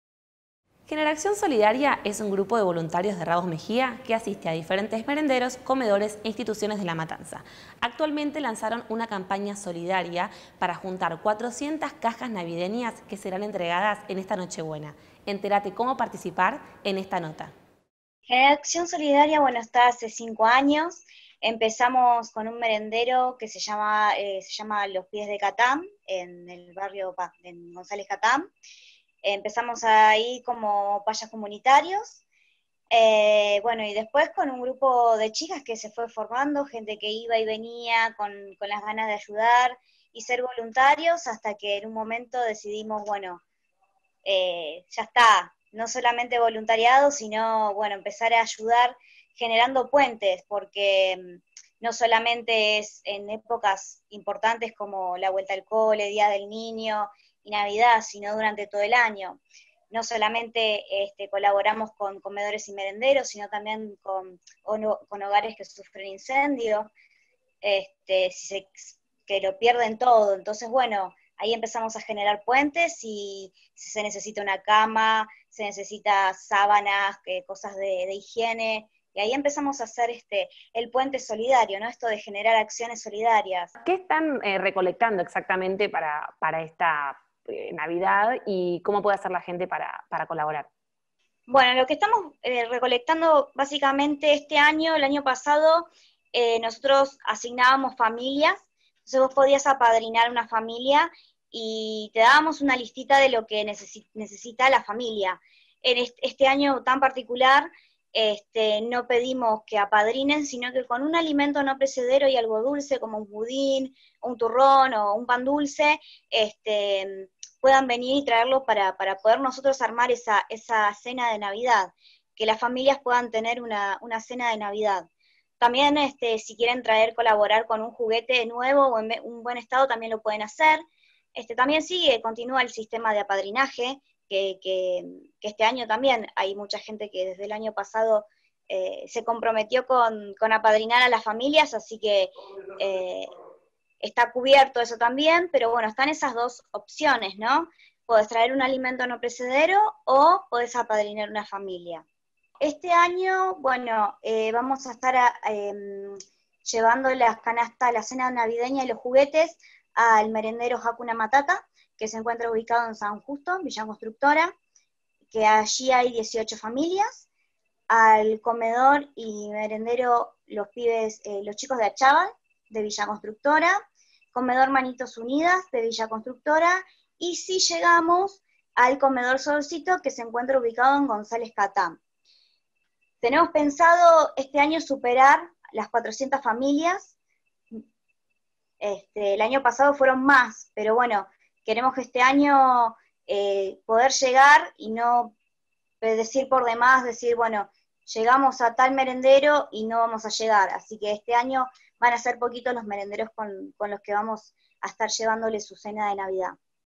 “GenerAcción Solidaria”, la organización de Ramos Mejía que lanzó una colecta navideña
El grupo GenerAcción Solidaria realiza una colecta, durante noviembre y diciembre, para llevar alimentos y regalos a las familias menos favorecidas del Distrito.

Allí reciben alimentos no perecederos, panificados dulces (como budines, turrones y pan dulce) y juguetes nuevos o usados en buen estado. La fecha máxima para aceptar las donaciones es el 20 de diciembre.

Quienes deseen colaborar, pueden acercarse a la Sociedad de Fomento “Amigos del Barrio Ingeniero Brian”, ubicada en Perito Moreno 1421, Ramos Mejía, de lunes a viernes de 13 a 15 hs. o comunicarse a los números: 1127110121 (Dulce), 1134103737 (Diamela), 11546267 (Alejandra).